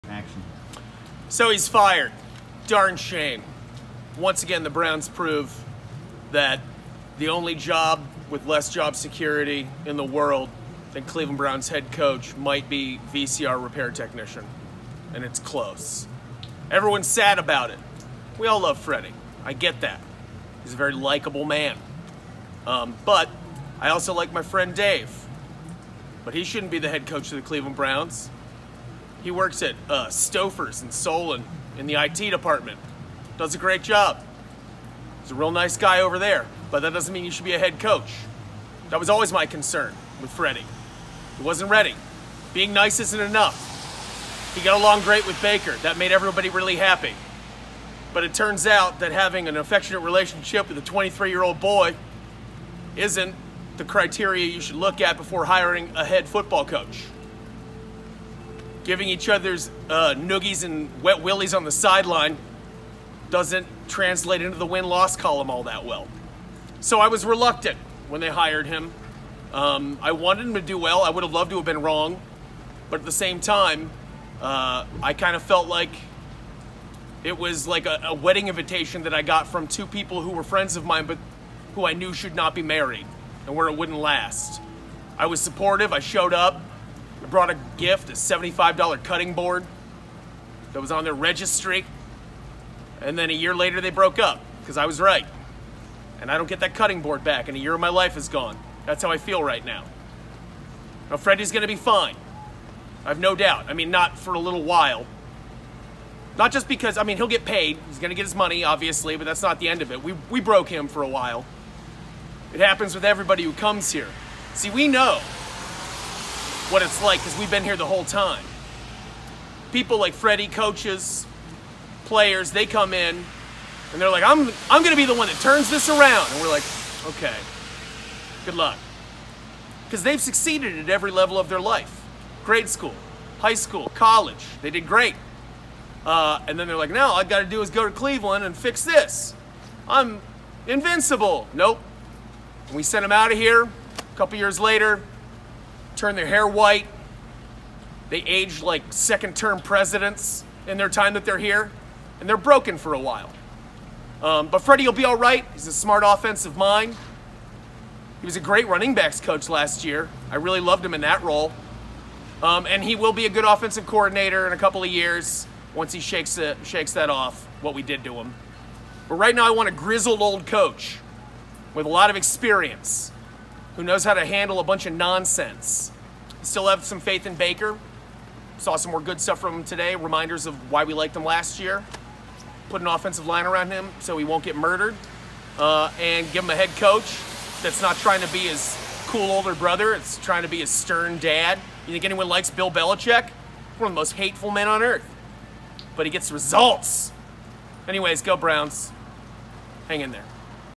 Mike Polk Jr. reacts to Freddie Kitchens' firing
The Cleveland Browns ended the 2019 season with a 6-10 record and search for a new head coach. Mike Polk Jr. reacts to the firing of head coach Freddie Kitchens.